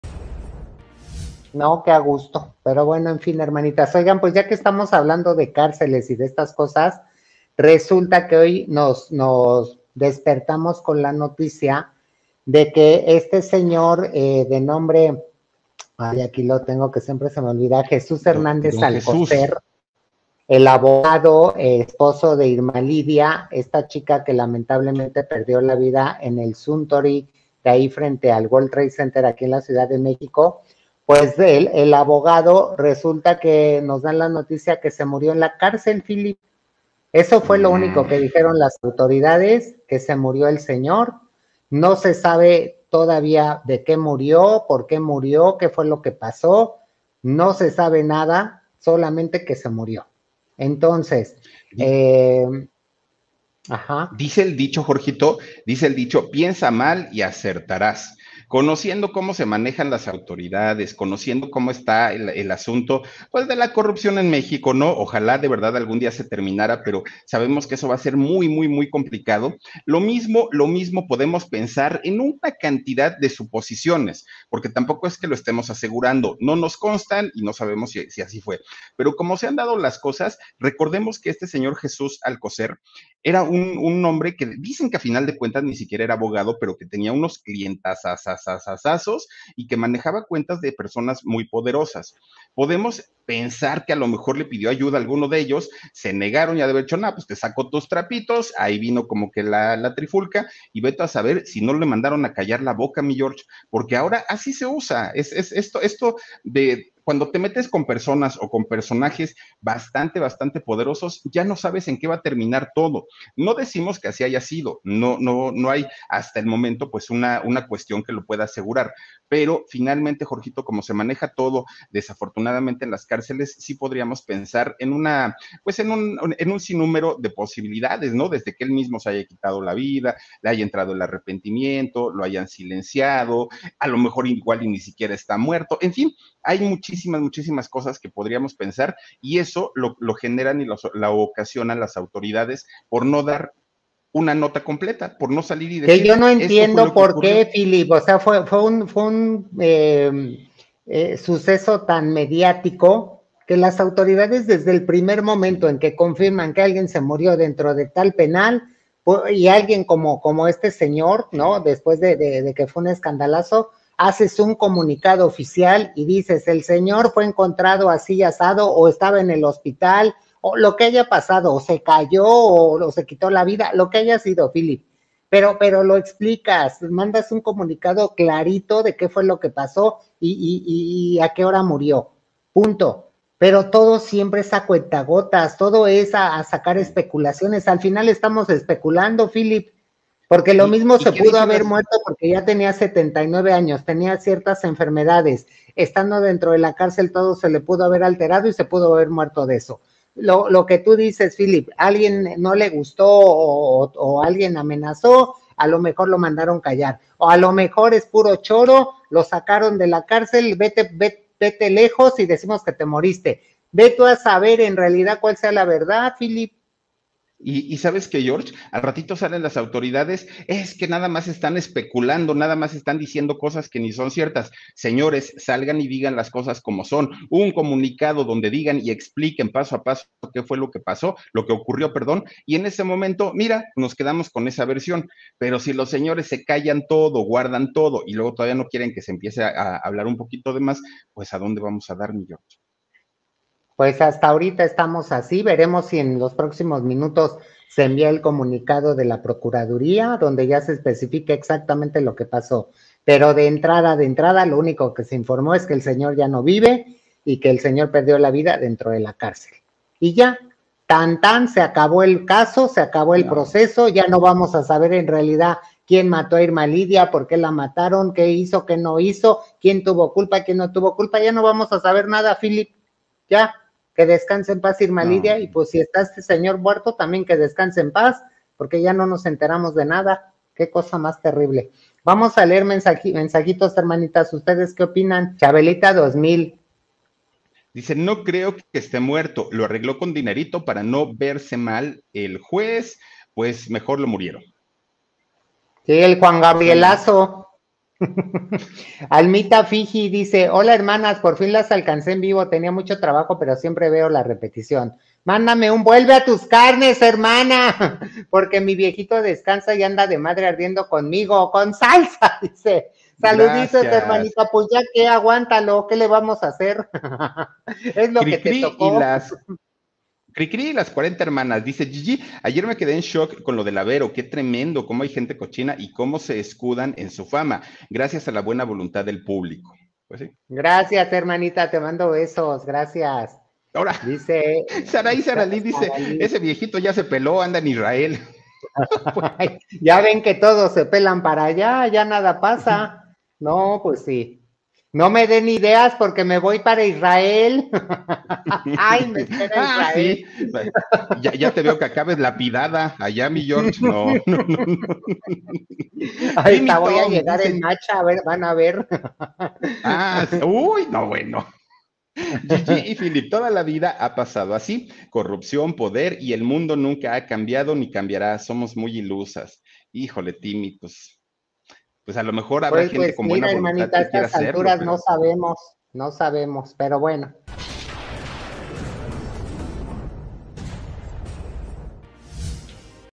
¡ TERMINA LA INVESTIGACION SOBRE YRMA LYDYA !
Hoy te contamos, todo lo que sucedió con el esposo de la joven cantante Yrma Lydya, quien lamentablemente perdió la vida en un restaurante de la Ciudad de México.